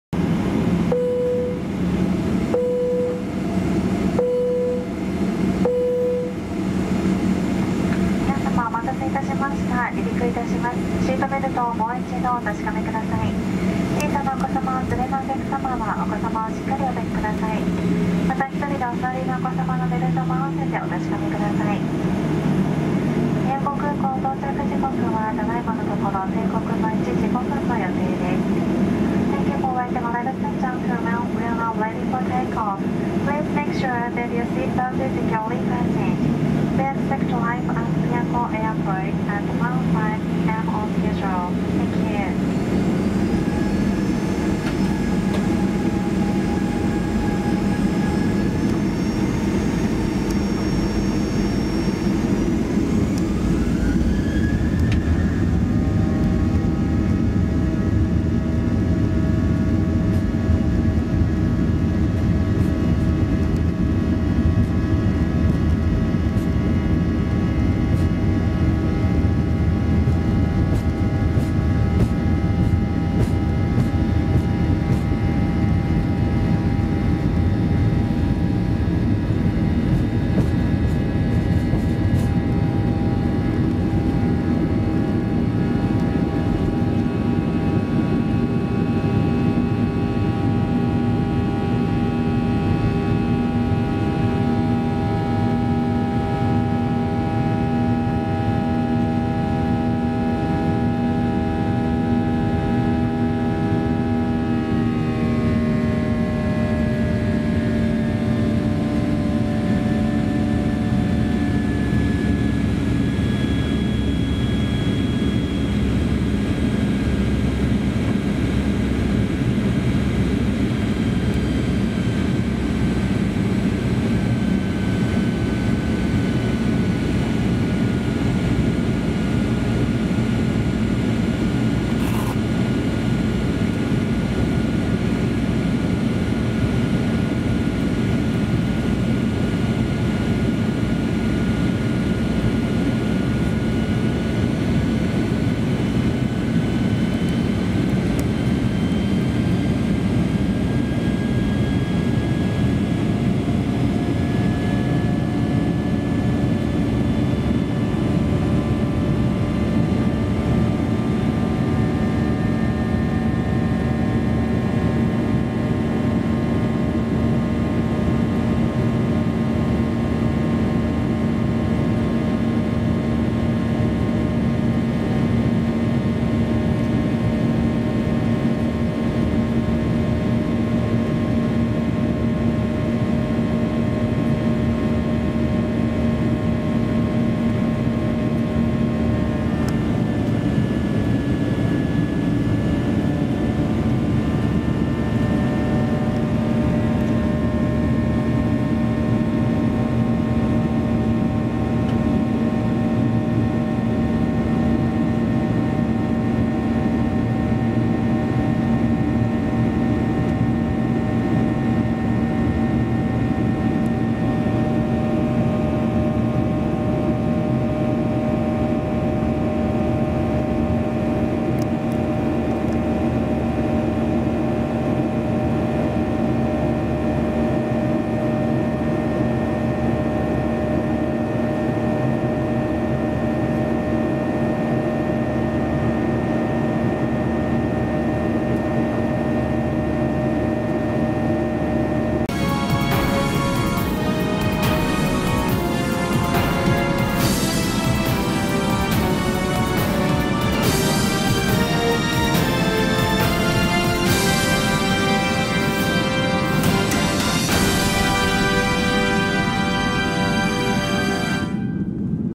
那覇空港RWY36離陸～那覇市街地／ANA那覇空港→宮古空港プレミアムクラス
ANA1725那覇空港→宮古空港B737-800(JA57AN)のプレミアムクラスより撮影した那覇空港RWY36離陸～那覇空港ターミナル上空～那覇市街地映像です。

Everyday Update!